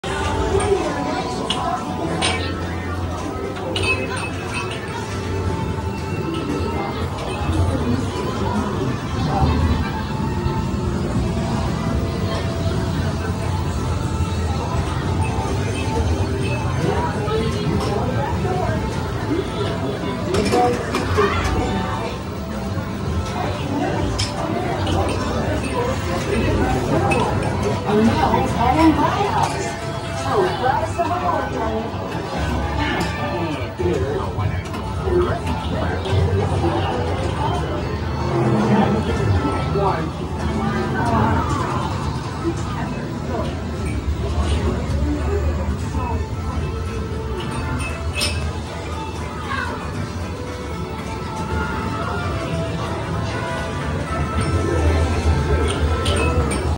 Some Classic Arcade games at Leaderboard Arcade & Bar in Jacksonville Florida
My camp friend Ethan saw this